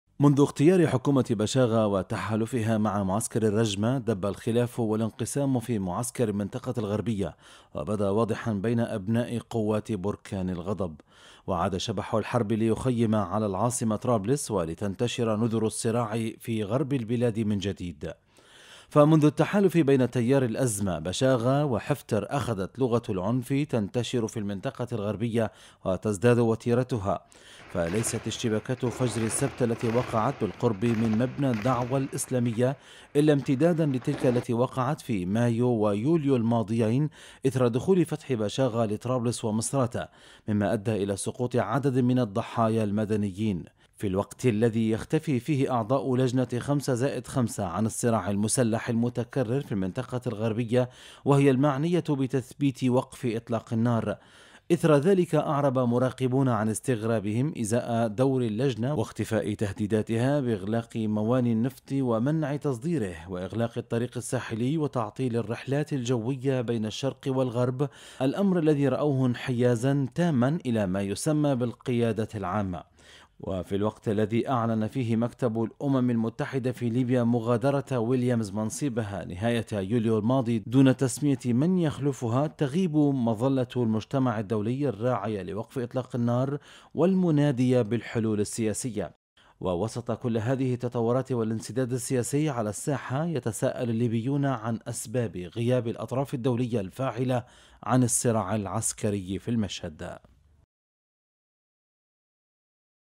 تقرير - تغطية إخبارية | الحكومة الموازية..وعد بالتداول السلمي، وتهديد بالحل العسكري | 07 - 08 - 2022
ــــــــــــــــــــــــــــــــــــــــــــــــــــــــــــــــــــــــــ
يمكنكم تحميل تطبيقنا TANASUH TV على الهواتف الذكية: